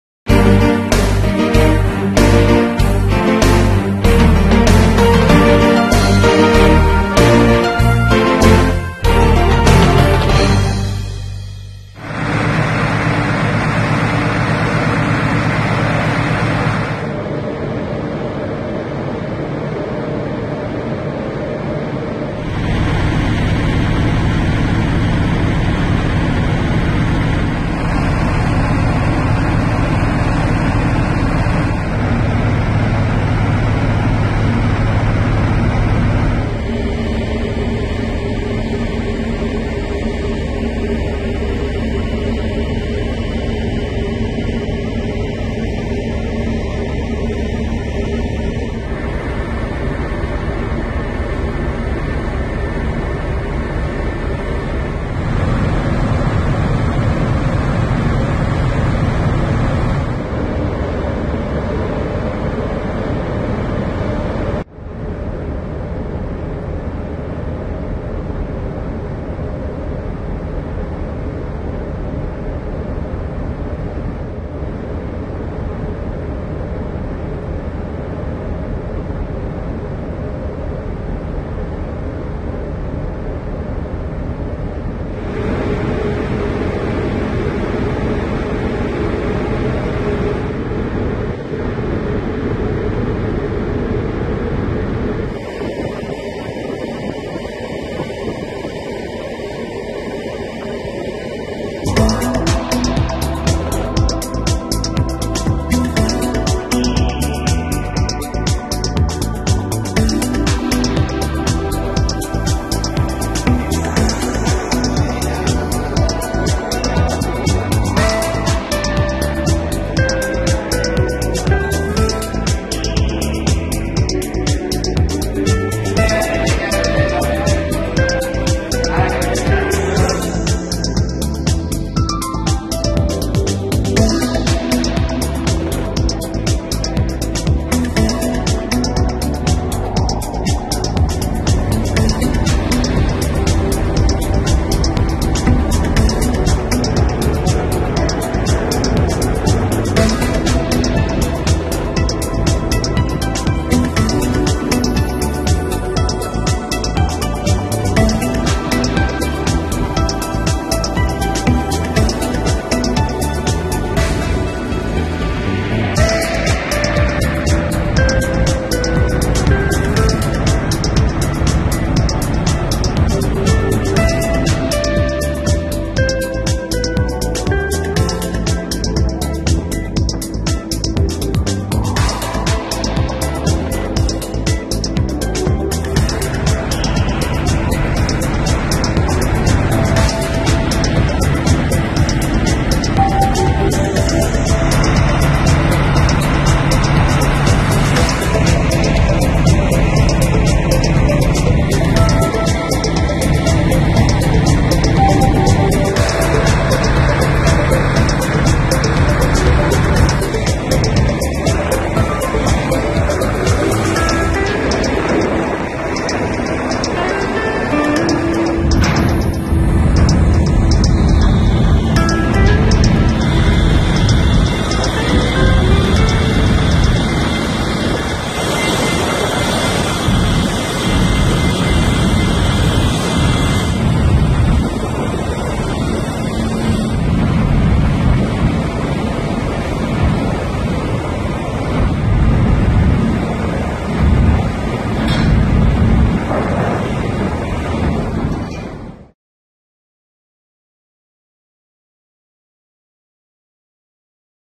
Passage ferry ABEL MATUTES, Barcelona - Palma de Mallorca (Balearia)
A nightrip from Barcelona to Palma de Mallorca.
Balearia
04/2016
4K

►BTC: bc1qk7d8qmrde35dxv42y0l5g4xxyd4q6q3hllgp65
►ETH: 0xE2b477CE35eA2e0BB28ea6814264E41443b7dFaC
►DOGE: D6RWdeeV2Da7KUBweL4736Ag61zsguVFBR